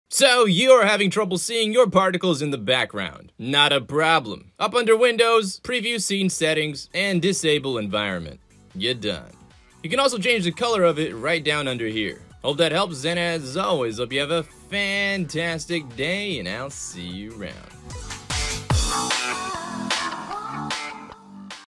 Unreal5 Niagra VFX: Disabling Preview Background
Here's how you can disable the background preview in the Niagra Particle Preview in Unreal5-!!